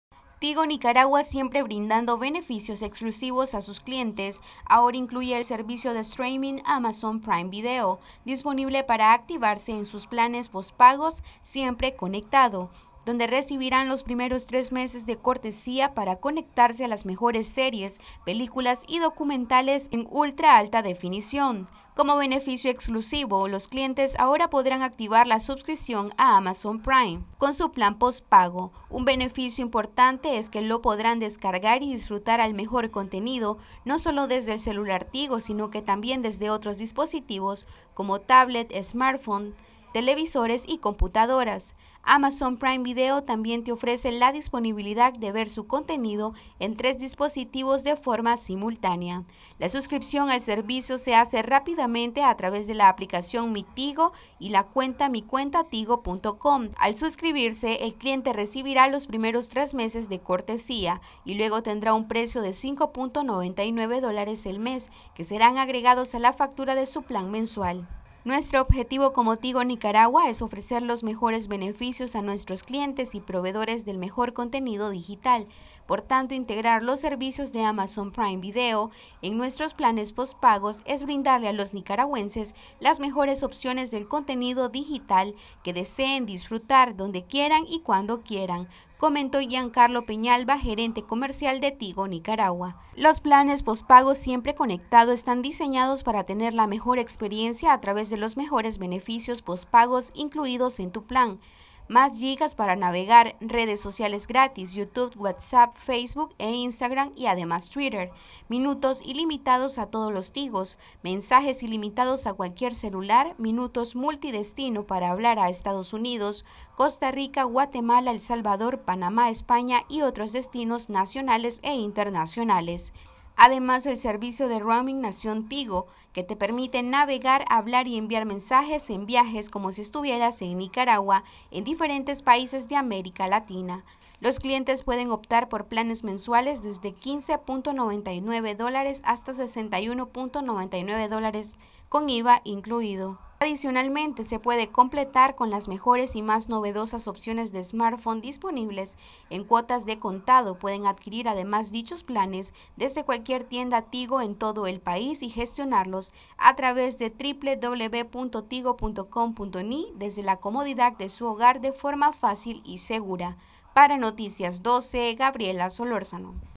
Amazon Prime Video con tus planes pospago siempre conectado de Tigo Nicaragua
Noticias12Nicaragua Tigo Nicaragua ahora incluye el servicio de streaming Amazon Prime Video disponible para activarse en sus planes pospago